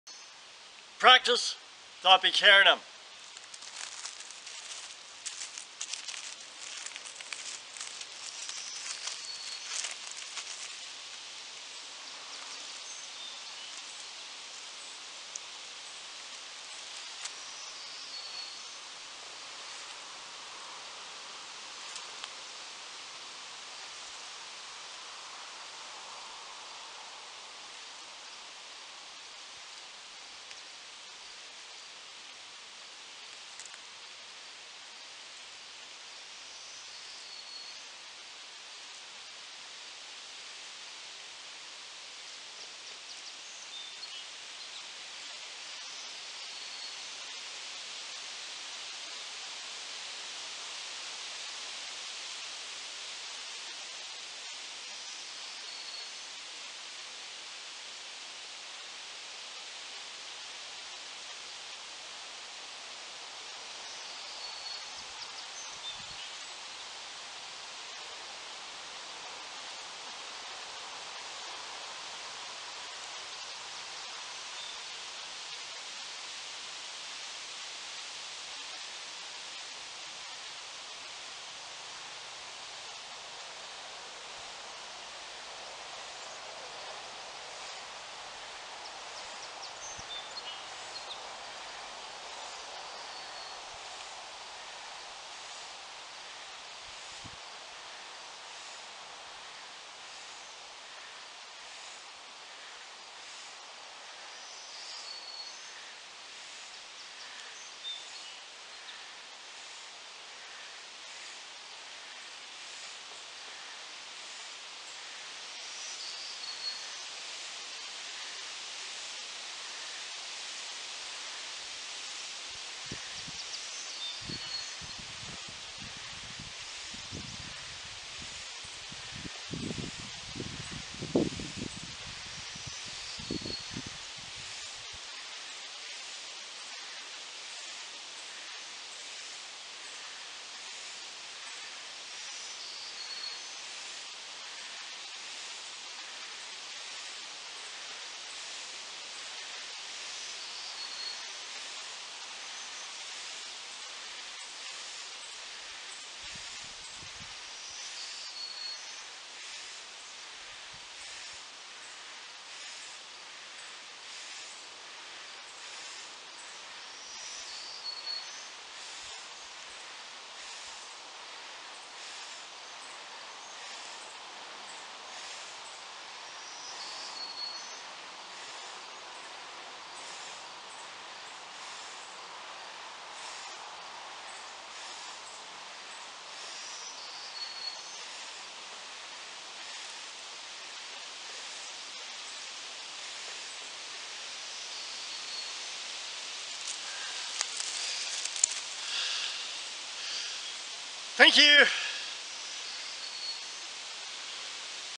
Thoppukaranam, demo 1103
Practice Thoppukaranam. With your left hand, grasp your right ear lobe, thumb forward, with your right hand, grasp your left ear lobe, thumb forward. Squat as deeply as comfortable, inhaling through your nose on the way down, and exhaling on the way up.  There are benefits if you face east and plant your tongue on the roof of your mouth.